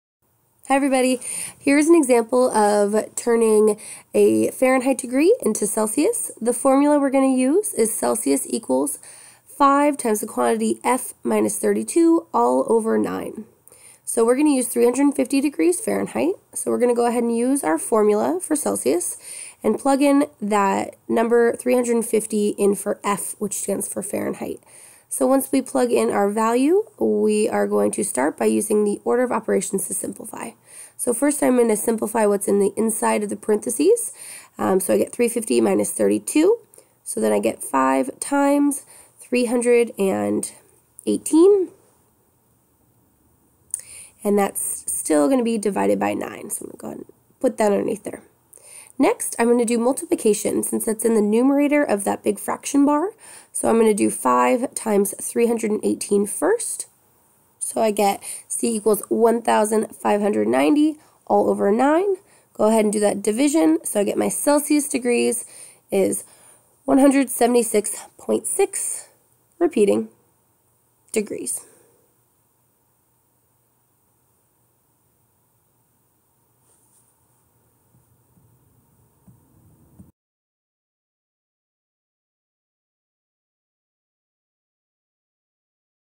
Convert Fahrenheit to Celsius- How to use the Celsius formula to convert
This quick example shows you how to convert 350 degrees Fahrenheit to Celsius.